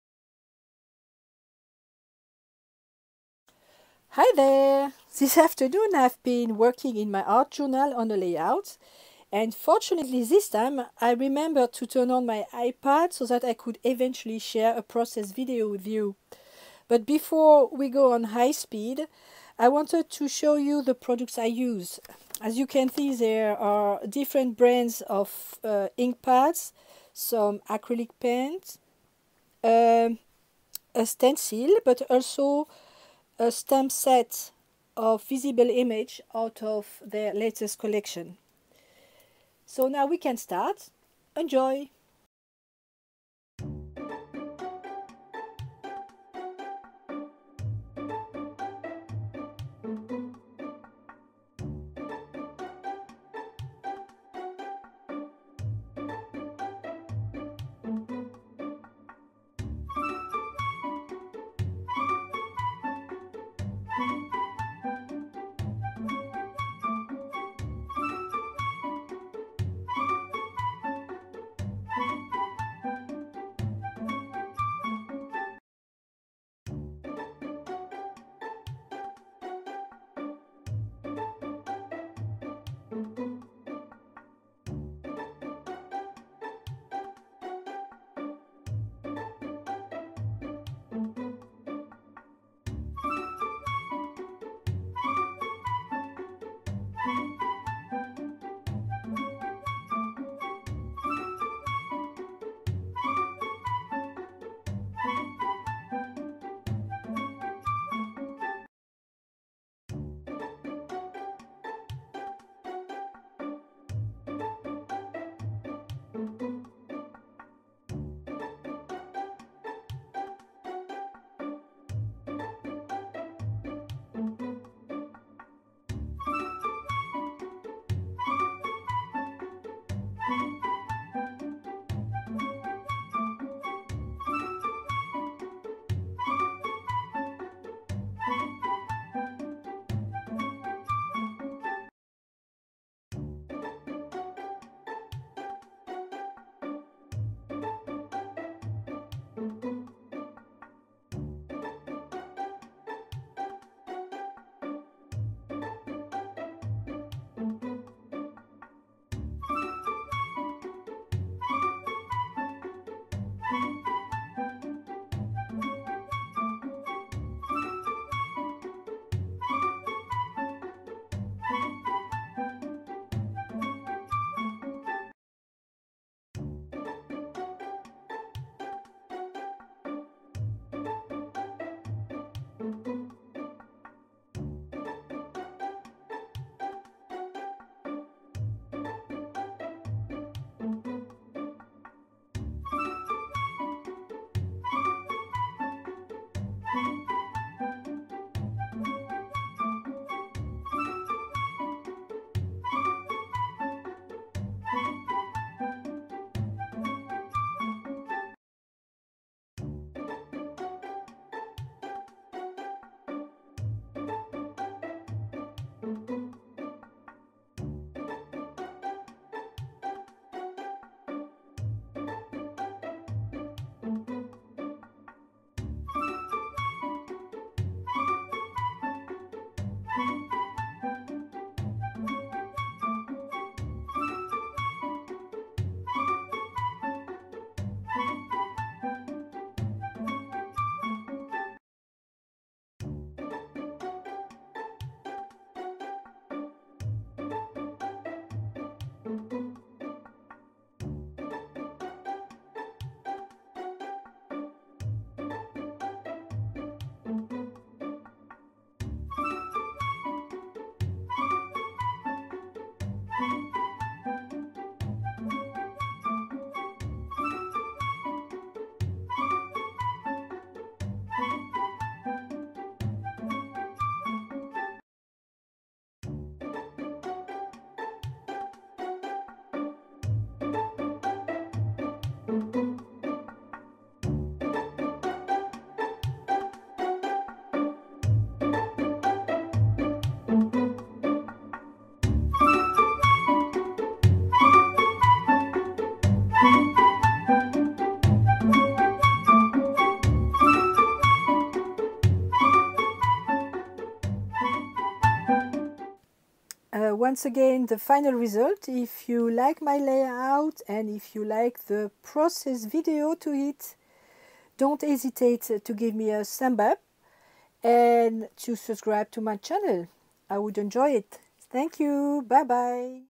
Process video to my layout with a stamp set by Visible Image
I received lately the stamp set “Random Art of Kindness” out of the new collection of Visible Image.
To try it out, I made a lay out in my art journal.
The other supplies I used:
Archival Ink pad- orange blosson and jet black
Distress Ink pad- mustard seed
Versafine ink pad - majestic blue
Dina Wakley Media acrylic paint - peacock, turquoise, penny, lemon and cheddar
Deco Art Media, fluid acrylic - ultramarine blue
Royal Talens Amsterdam - zink white
Shawn Petite Doodle mini stencil mask